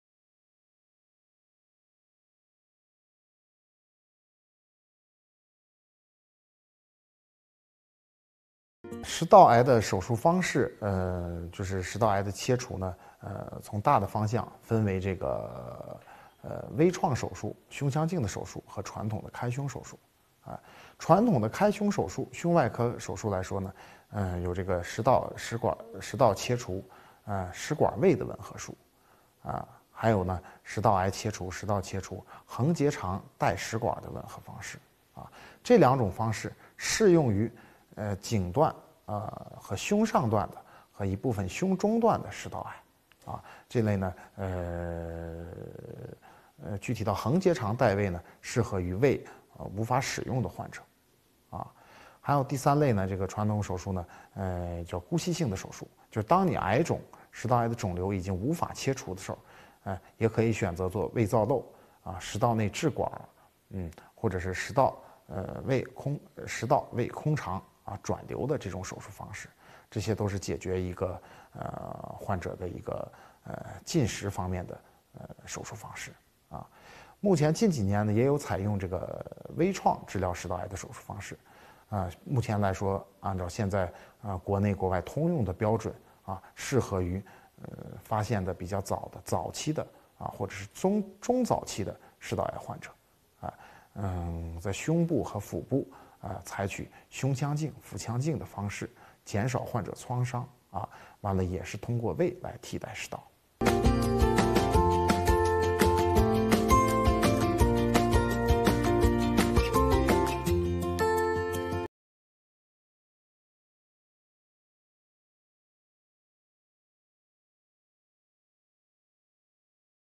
食道癌的手术方式有哪些 郑康 山西省肿瘤医院
食道癌的切除从大的方向分为：微创手术、胸腔镜手术和传统的开胸手术。传统的开胸手术有：食管胃的吻合术、横结肠代食管的吻合方式，适用于颈段、胸上段、胸中段食道癌。这类具体到横结肠代胃，适合于胃无法使用的患者。还有第三类手术叫姑息性手术，就是食道癌的肿瘤已经无法切除的时候，可以选择做胃造瘘、食道内置管或者是食道胃空肠转流的手术方式。

What are the operation methods of esophageal cancer
The resection of esophageal cancer can be divided into minimally invasive surgery, thoracoscopic surgery and traditional thoracotomy. The traditional thoracotomy includes: esophagogastrostomy, transesophageal anastomosis with transverse colon, It is suitable for cervical, upper and middle thoracic esophageal cancer. Specifically, the transverse colon is used to replace the stomach, It is suitable for patients whose stomach cannot be used. There is a third type of surgery called palliative surgery, It's when esophageal cancer can't be removed, You can choose to do gastrostomy, esophageal tube or esophagojejunostomy.